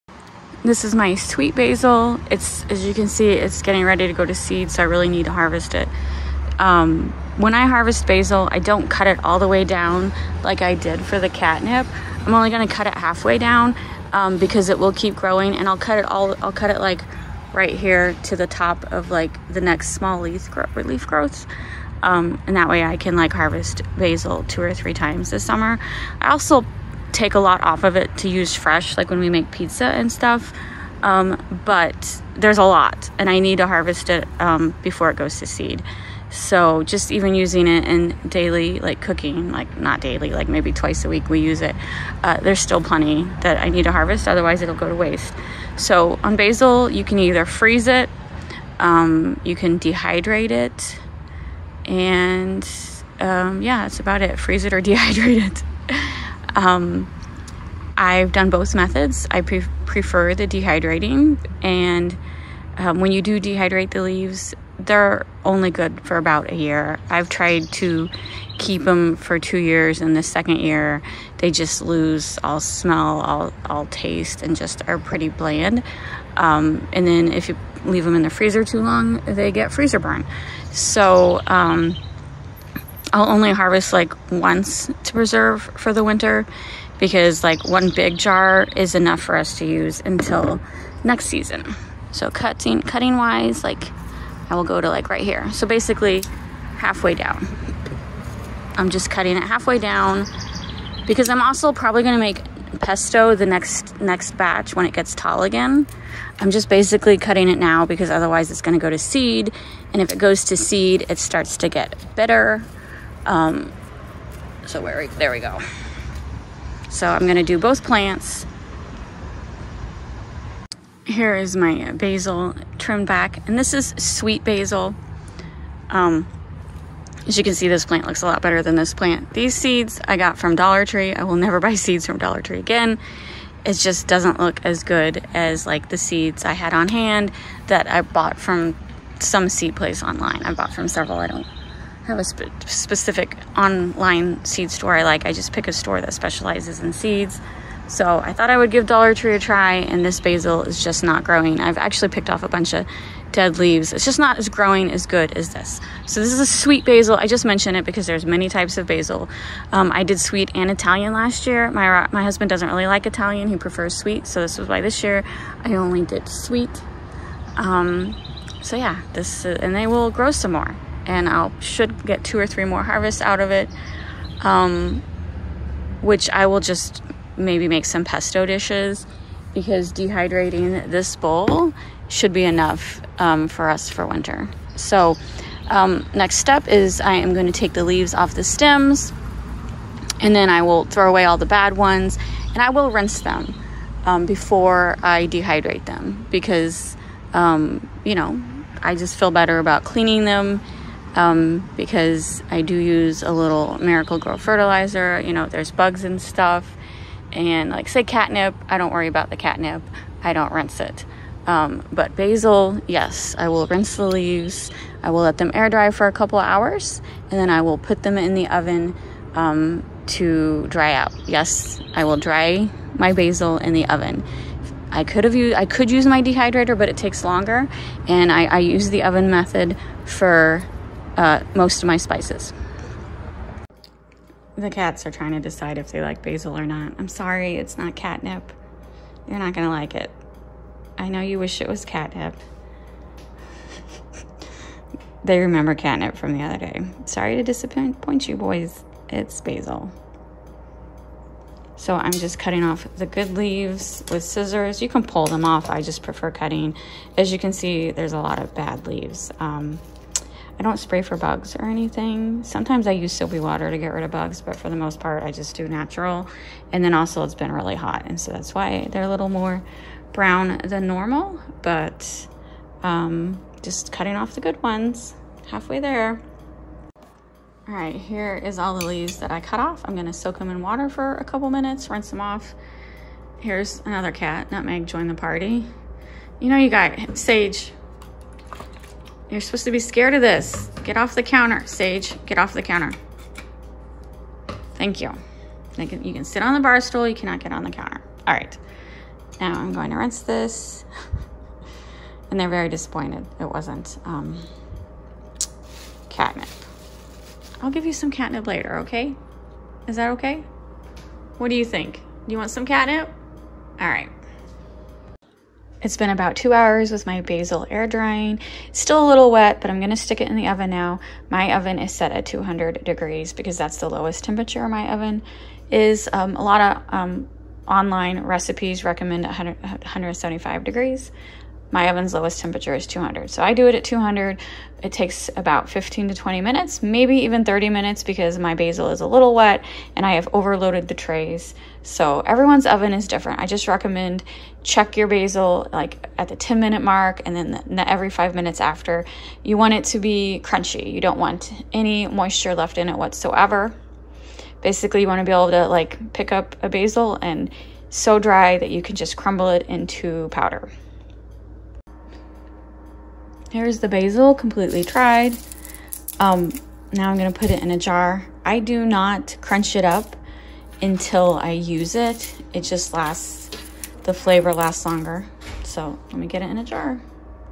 Growing Sweet Basil & Dehydrating For Long Term Storage
My favorite canning book: